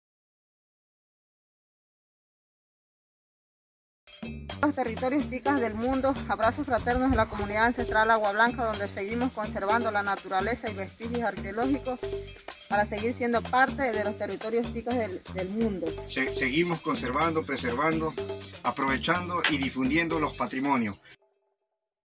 Agua Blanca | 16ª Asamblea General | Consorcio TICCA
Presentado por Carmen Merchan Gonzales y Paul Martínez Maldonado en nombre de Agua Blanca (ICCA en Ecuador) en la 16ª Asamblea General